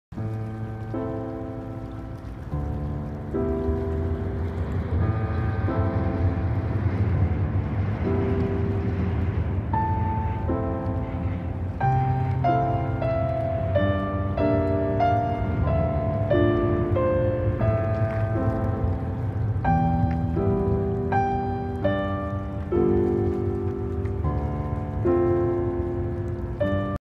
Danube river
An evening strolling down the Danube river hoping to catch the sunset.